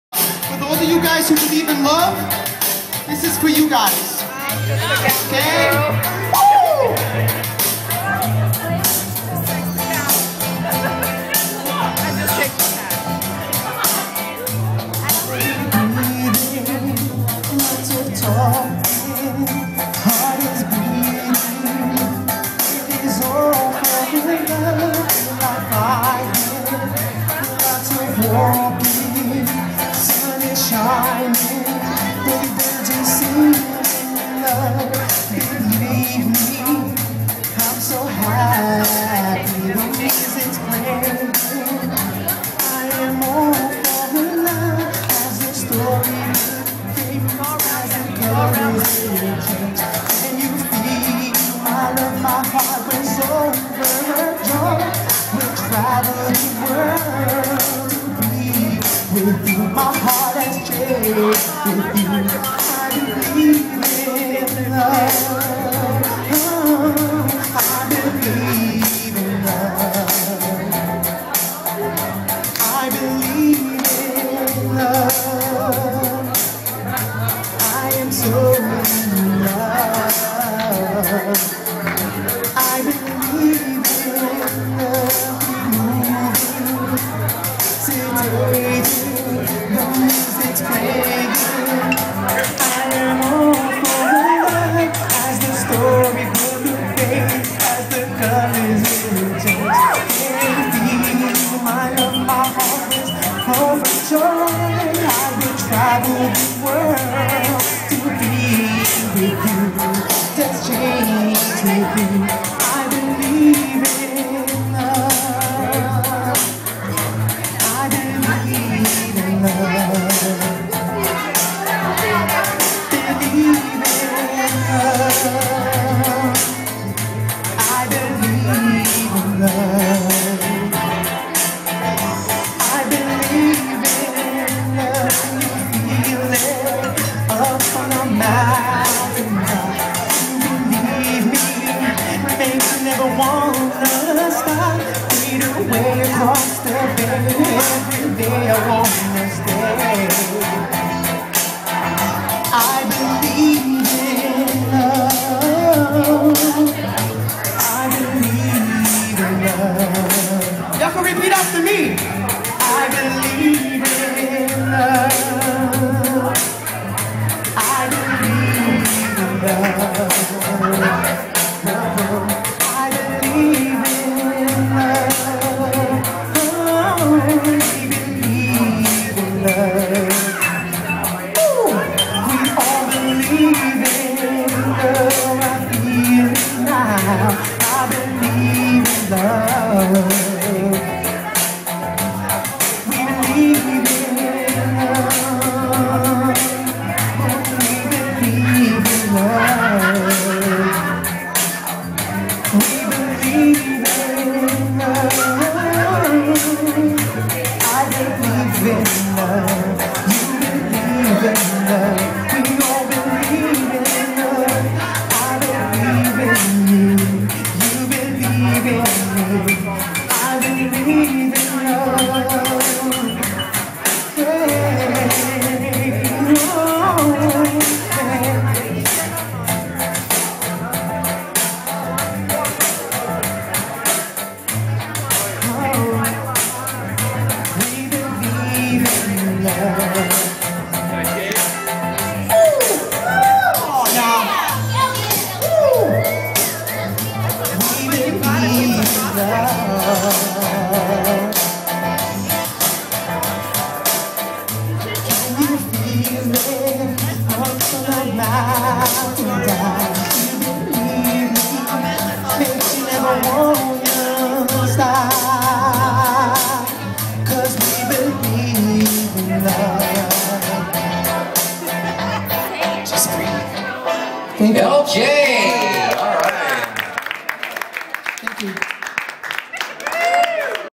Lj Ugarte Performing Live at the Kids Resource Event Part 3
Actor/Singer  Lj Ugarte Performing Live at the Kids Resource Redcarpet Event in Hollywood November 17th 2012